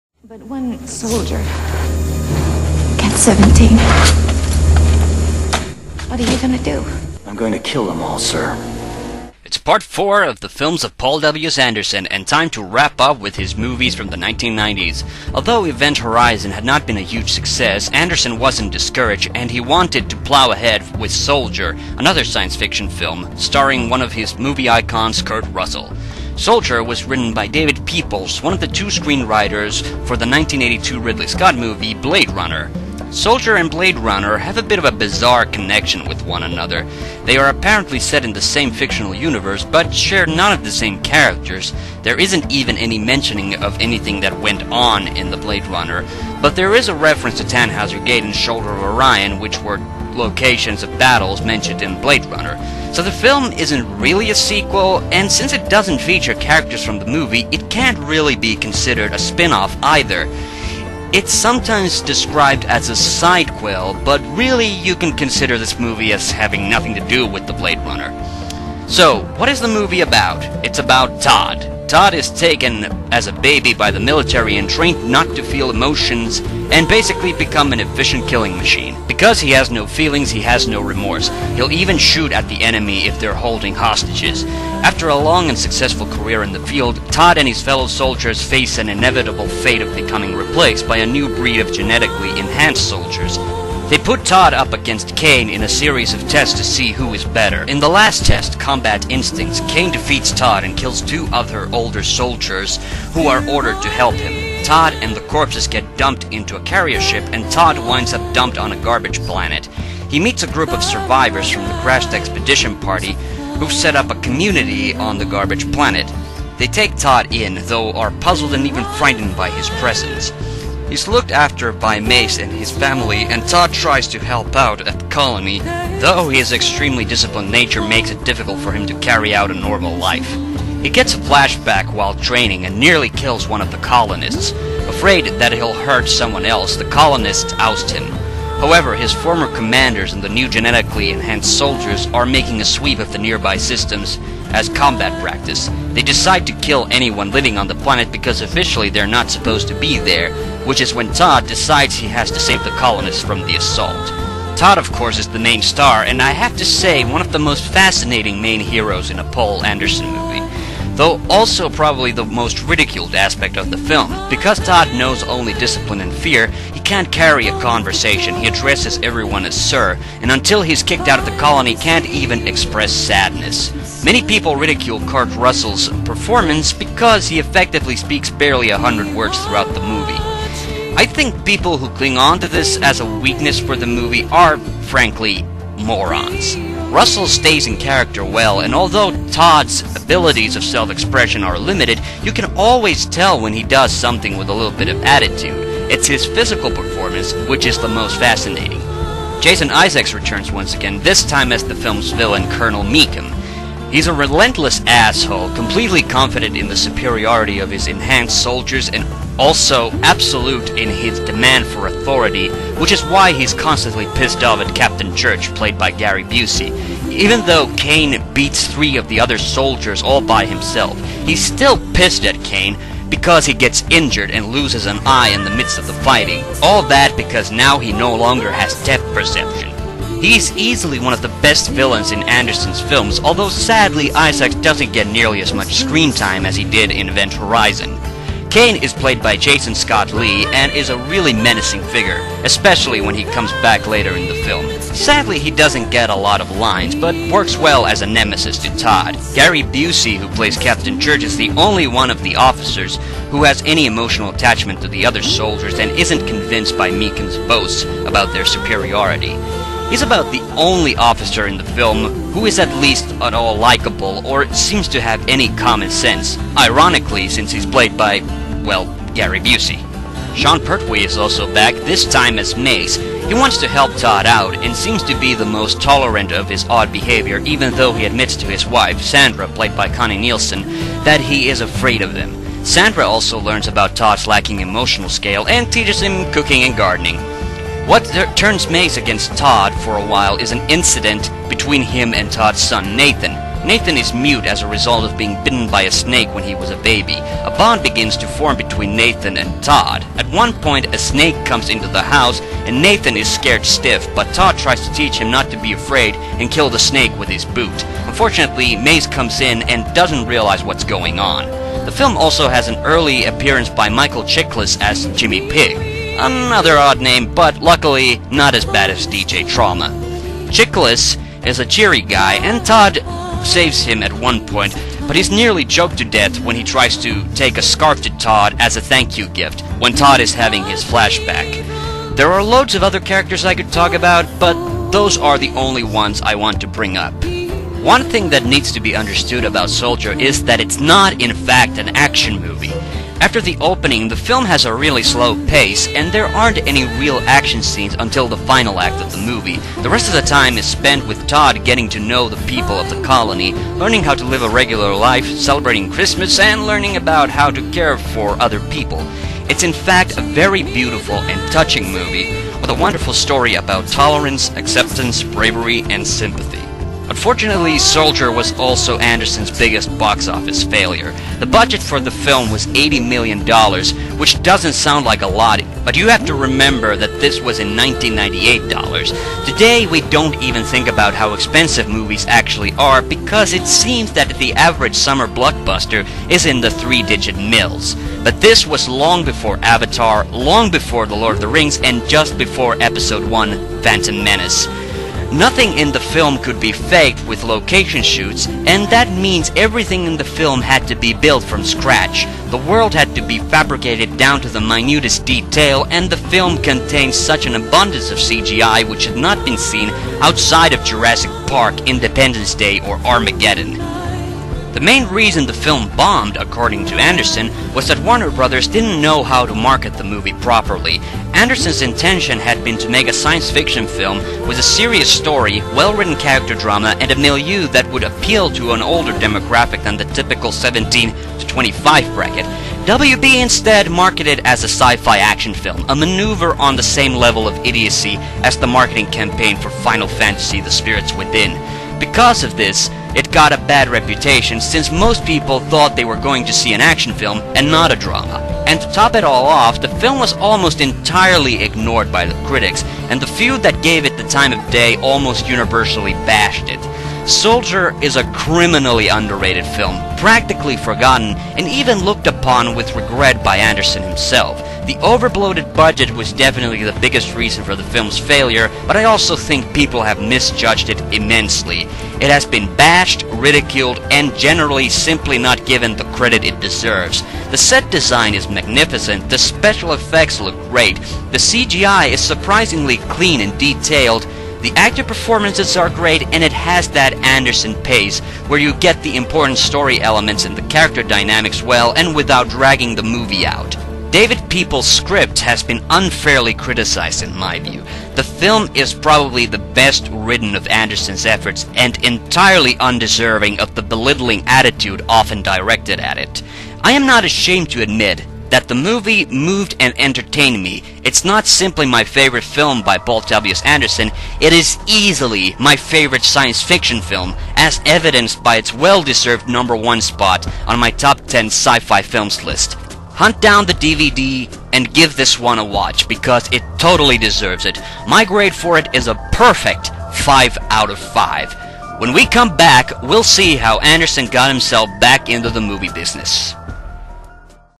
Review of "Soldier (1998)" - The Films of Paul W.S. Anderson Part 4
This episode was recorded with my new headset-mic so the audio quality isn't quite so good. I think I'll go back to using the old microphone for the next video.